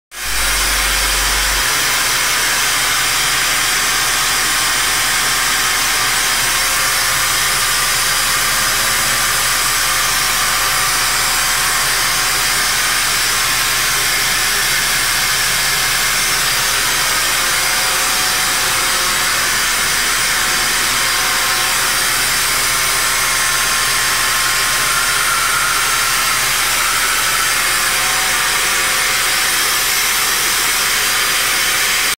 DIY Power Steering Flush Kit (part 2)
Power steering flush using a diy fluid vaccum made out of stuff found around the house (it works!)